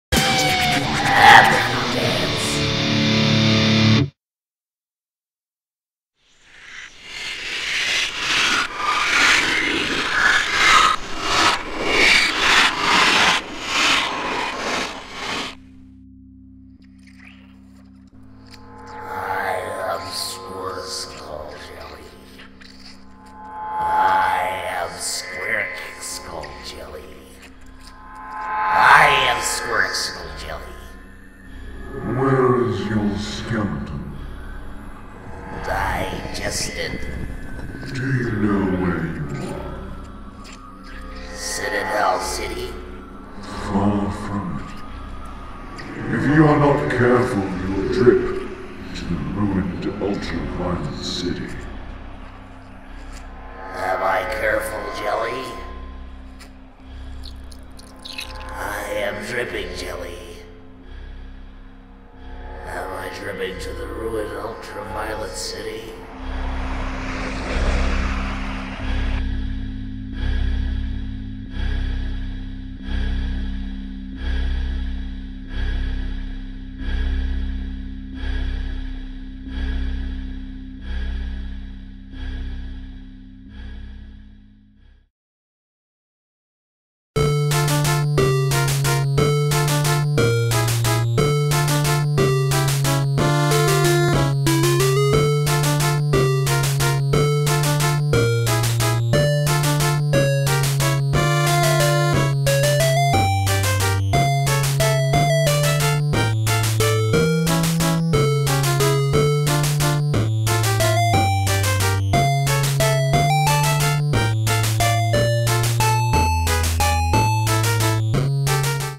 Skeleton Jelly: The Lost Chapter
A little extra bit of the Skeleton Jelly saga I forgot to voice, now for all of you little kiddies to enjoy! (Also sorry for the early upload, I had meant to schedule this for tomorrow, but Youtube sort of thought I DIDN'T set it as a scheduled video. Go figure!)

Skeleton Jelly and... Stone... Man... - Myself

Source comic: Matt Brinkman (unknown original source)